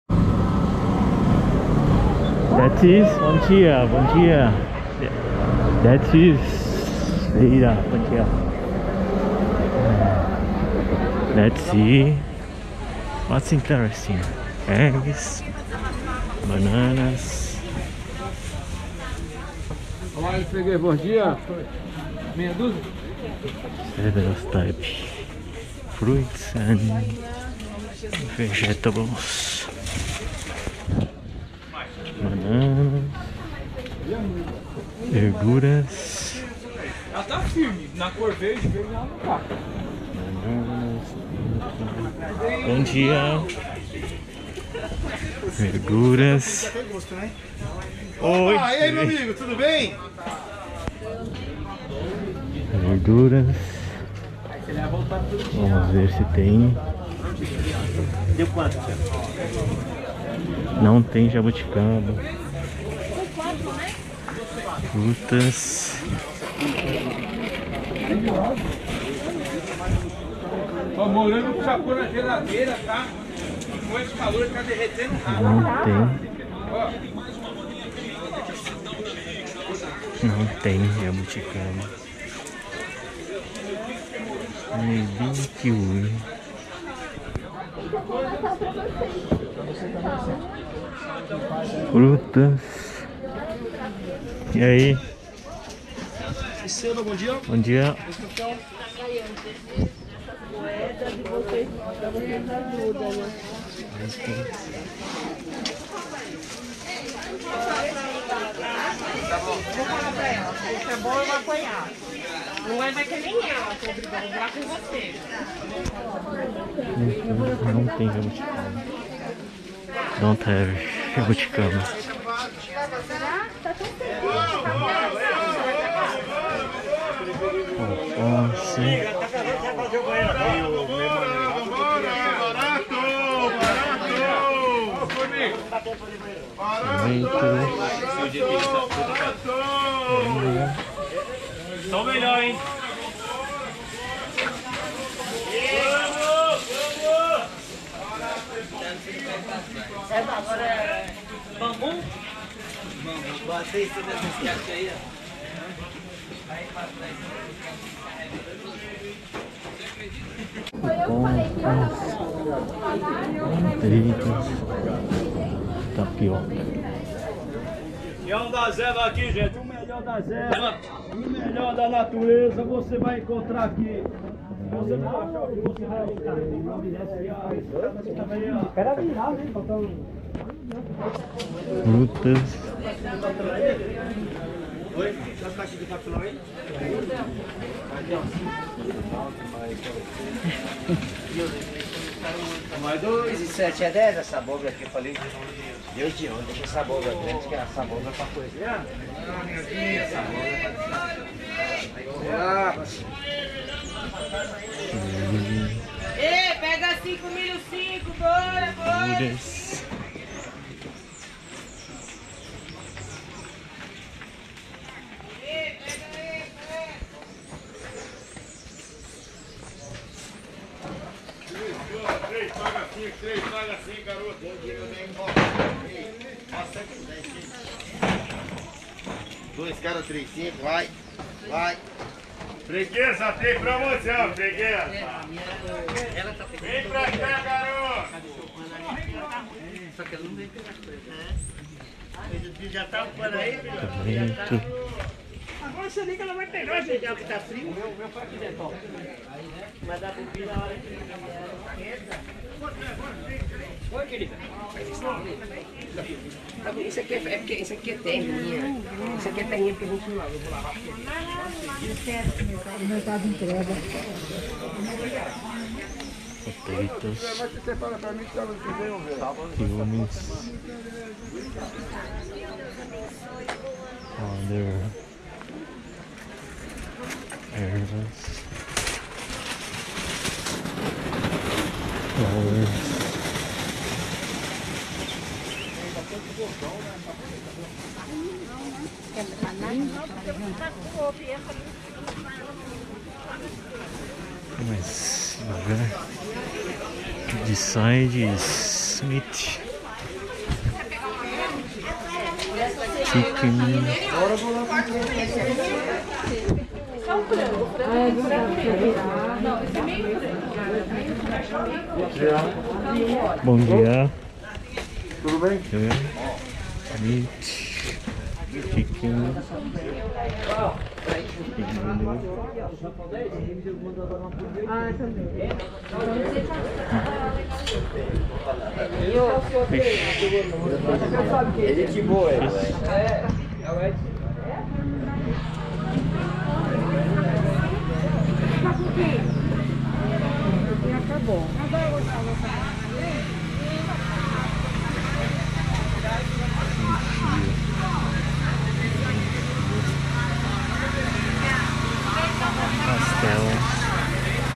2021-12-18 Feira Tremembé
Feira Tremembé

in Brazil, in almost all neighborhoods there are street fairs (feira-livre) that take place once or twice a week.
Look: